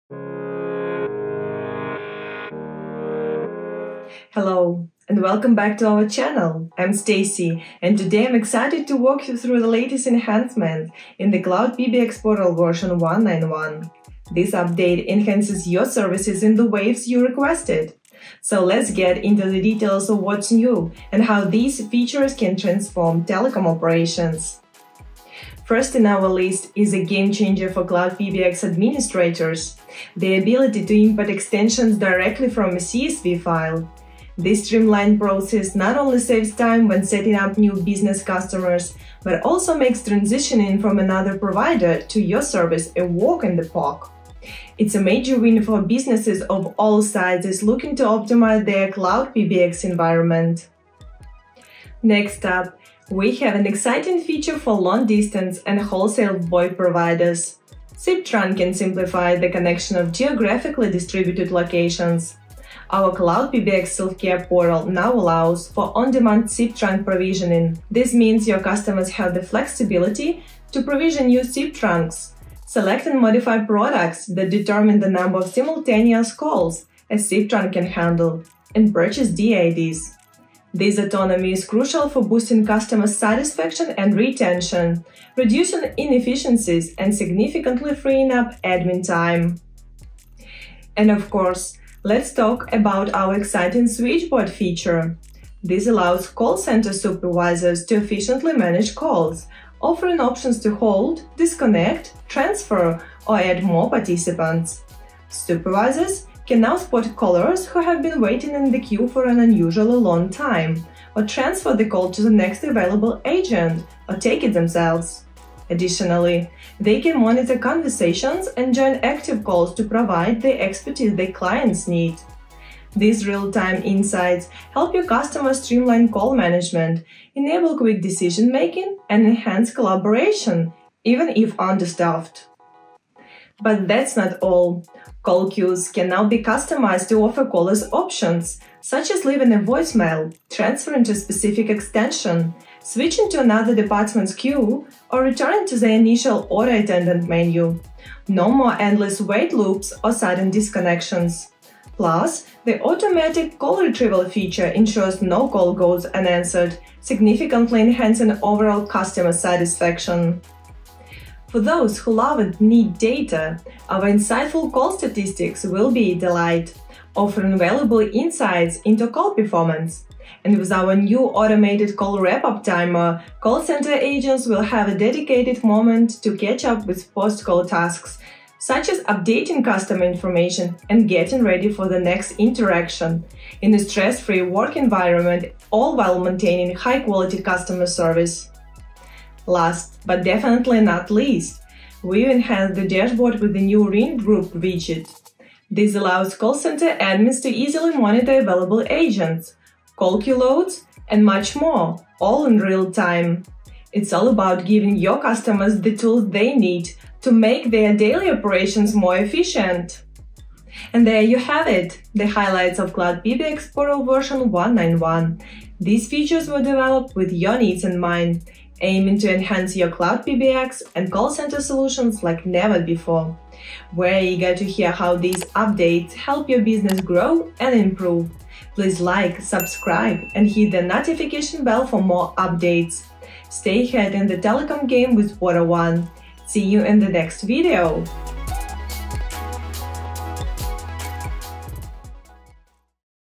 New Enhancements to the PortaOne Cloud PBX Portal | PortaSwitch + Cloud PBX | PortaOne News
PortaOne is pleased to introduce version 1.9.1 of our cloud PBX portal, which has new enhancements driven directly by requests from PortaSwitch and PortaBilling users. In this video, PortaOne technical writer Anastasiia Holota explains all the new features and benefits that are now available in this update, including the ability to import extensions directly from a CSV file, on-demand SIP trunk provisioning for long-distance and wholesale VoIP providers, a better call-queue experience, enhanced call center supervisor management features, more detailed call statistics, an automated call wrap-up timer, and a new dashboard widget that allows admins and supervisors to easily monitor available agents, call queue loads, and more.

Timecodes:
00:00 Introducing the PortaOne cloud PBX portal v. 1.9.1
00:26 Effortless import of extensions directly from CSV files
00:52 Flexible cloud PBX and SIP trunking management
01:30 Enhanced call control for call centers
02:13 Exit from call queue options for callers
02:41 Insightful call statistics and automated wrap-up scheduling
03:09 Dashboard enhancements
03:30 An invitation to connect with PortaOne